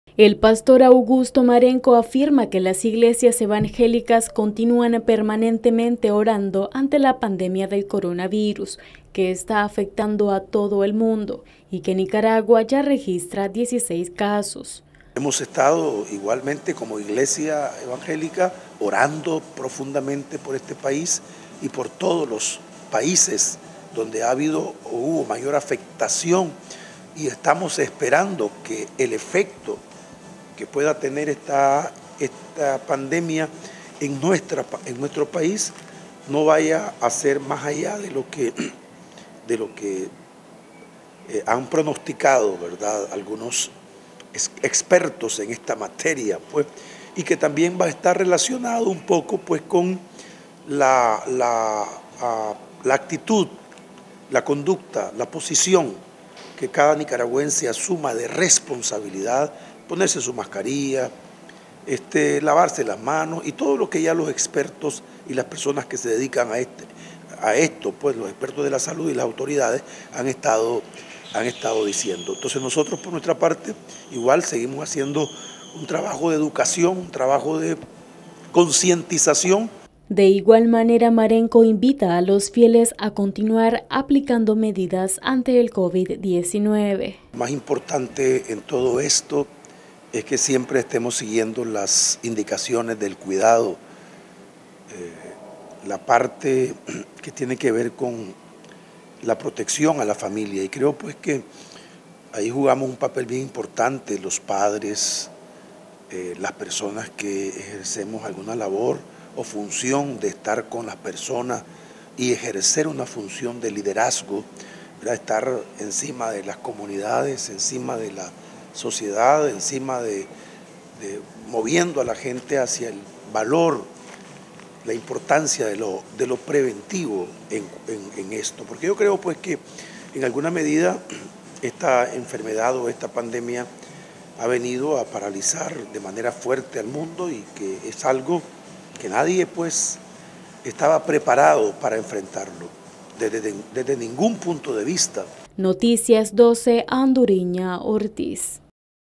Iglesias evangélicas mantienen oración por Nicaragua ante COVID-19
El pastor Augusto Marenco afirma que las iglesias evangélicas continúan permanentemente orando ante la pandemia del coronavirus que está afectando a todo el mundo y que Nicaragua ya registra 16 casos.